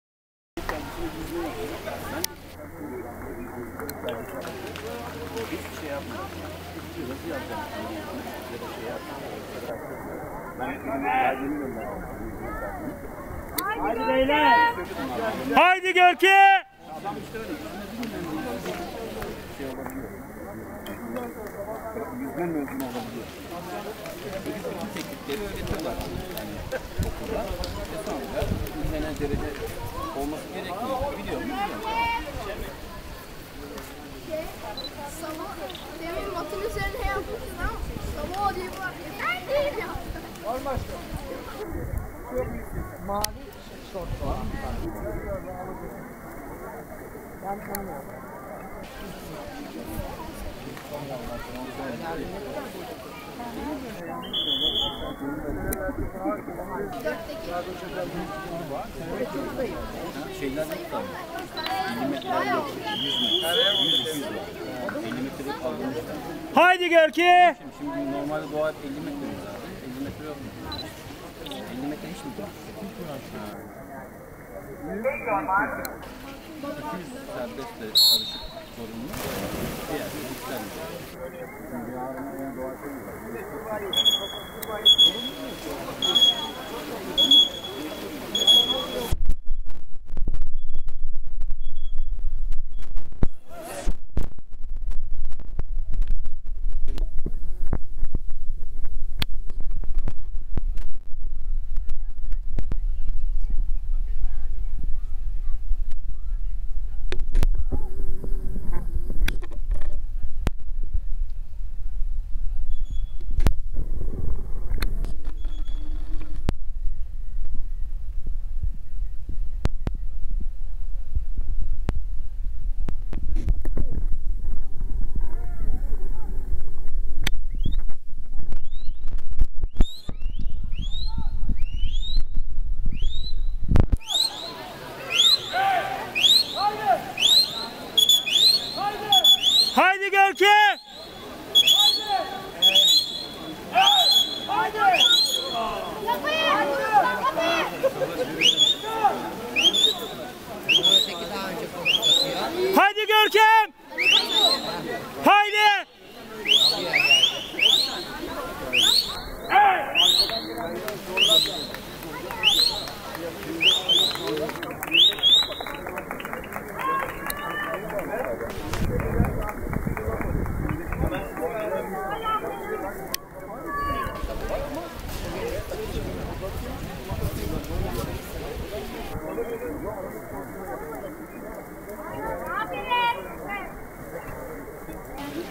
100m breaststroke 1.33 min Olimpik Kulaclar İzmir 24 Temmuz'15
Olimpik Kulaclar izmir yarışları.. 100m Kurbağa  bir üst yaş gurubuna karşı ...4ncülük..